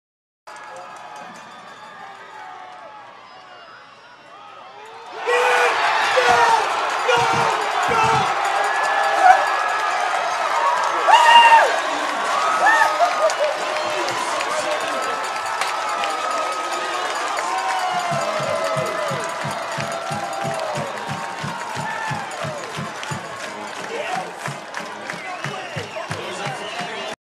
Treception
Udofia Pick 6 for the win against Vista Murrieta